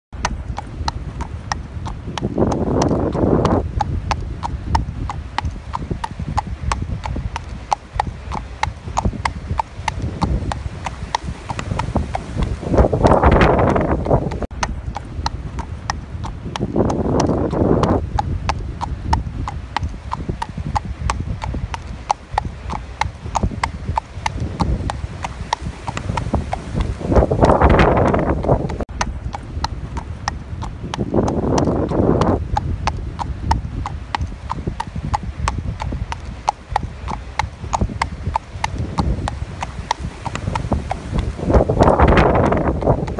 Dachshund Takes Job of Walking Horse Very Seriously
Credit: Dominic Buckley via Storyful